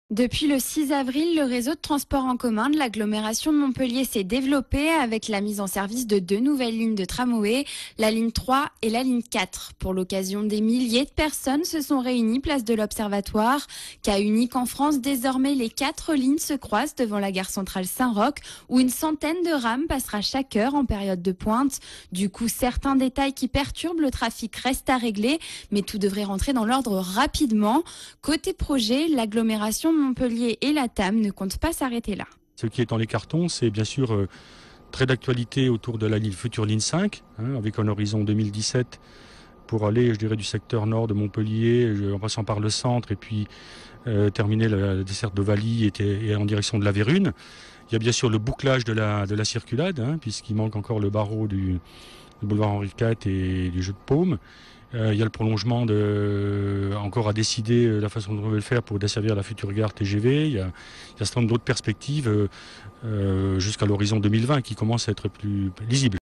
Montpellier étend son réseau de transports en commun
Sites webs/blogs, copiez cette vidéo avec le bouton "Intégrer" !
Le réseau de transports en commun de l'agglomération de Motpellier se développe avec la mise en service des lignes de tramways 3 et 4. Pour exploitation TV, droits réservés, nous contacter